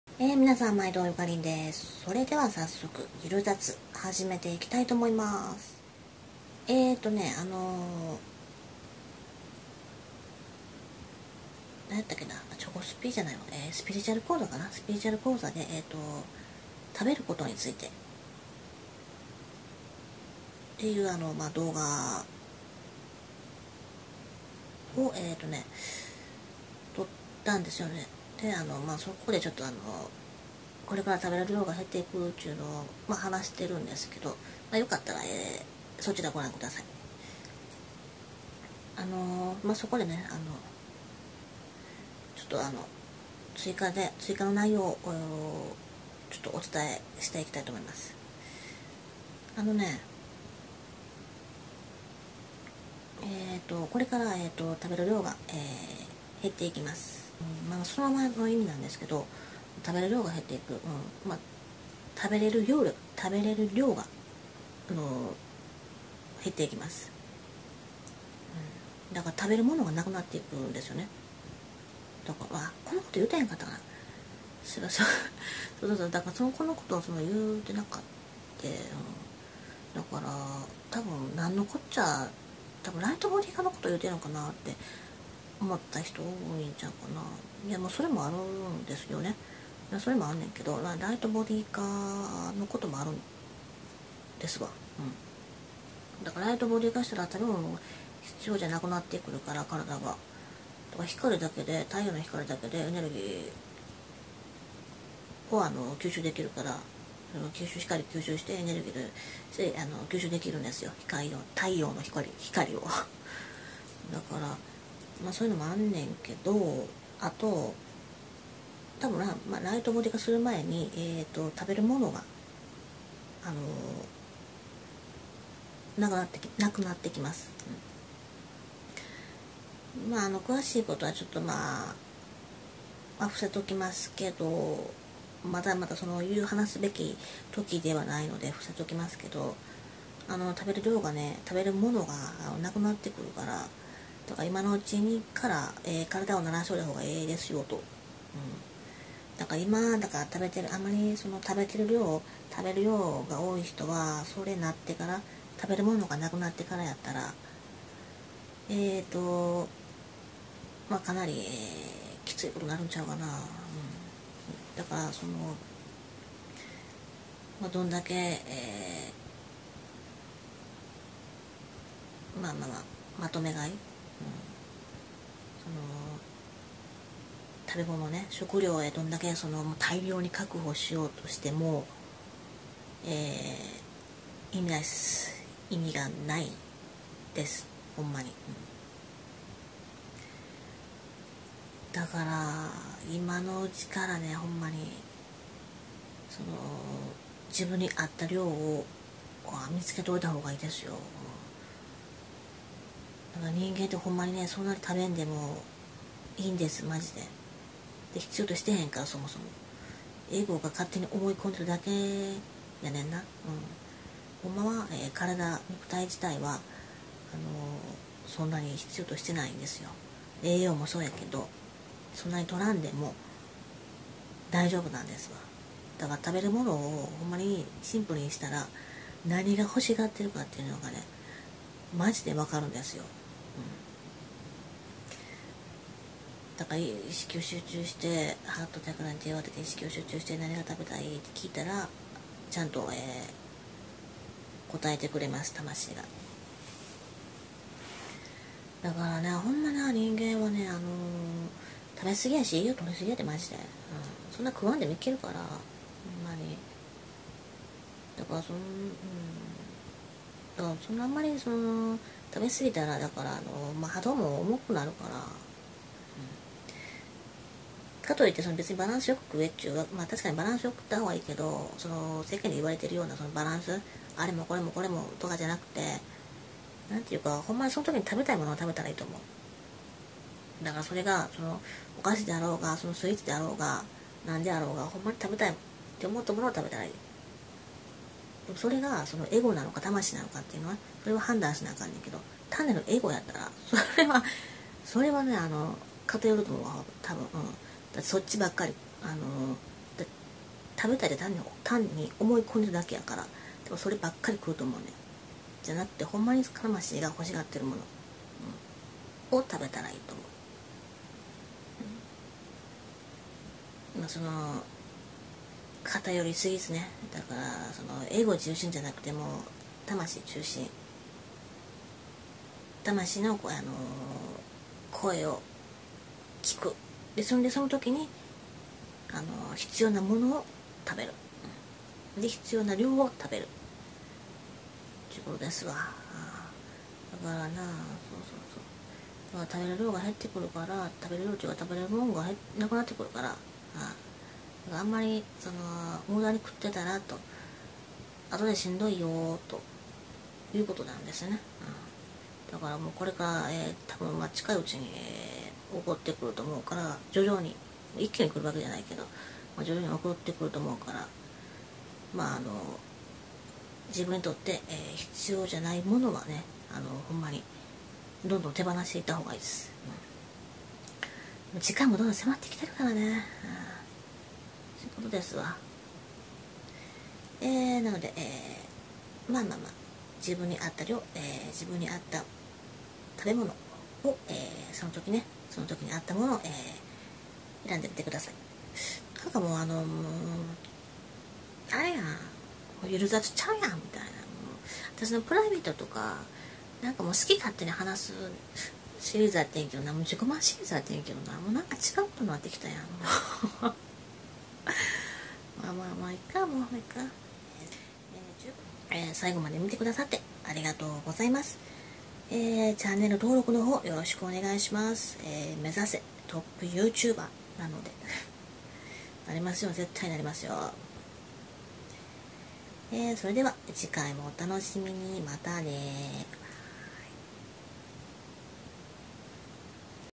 【スピリチュアル講座】この先、食べる物がなくなる！？食糧難？！＃17
今回は、この先、食べる物がなくなる？！について語ってみました。実はこの先、食べる物が・・・・信じるか信じないかは、あなた次第！？